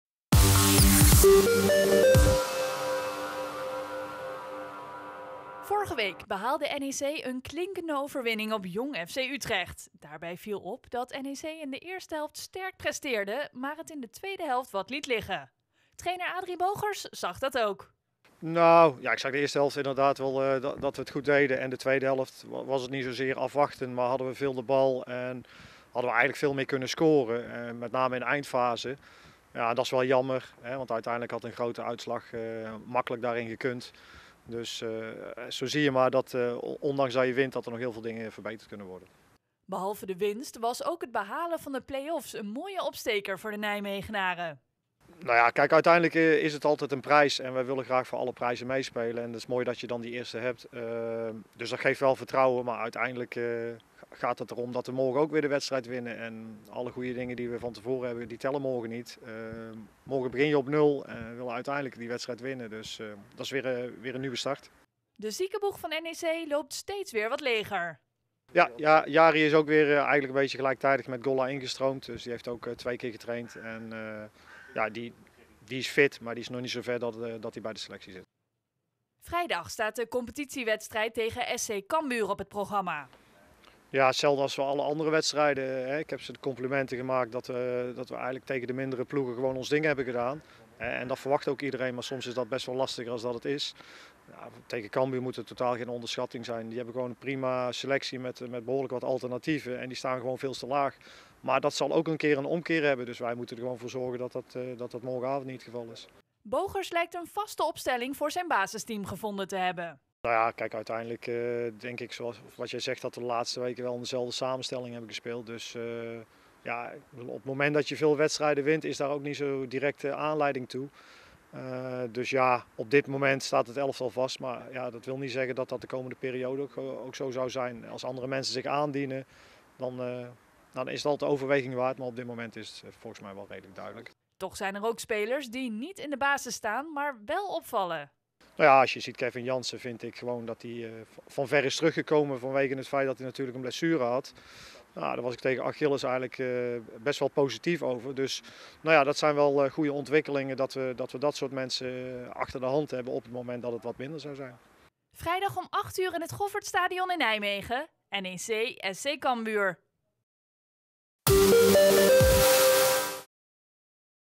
Voorbeschouwing: NEC kan 'concurrent' op grote achterstand zetten
NEC speelt vrijdagavond tegen SC Cambuur. Voorafgaand aan het seizoen leek het een duel met een andere titelkandidaat, maar NEC kan de Friezen al op zeventien punten zetten.